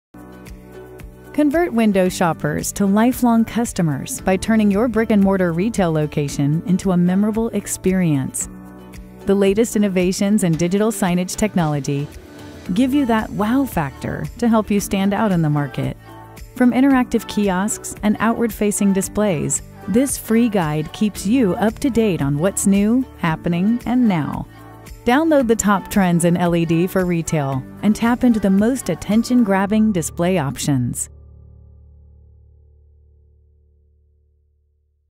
Retail’s Biggest Digital Display Trends
Our latest guide explores the top trends in retail digital signage, courtesy of the experts at Samsung.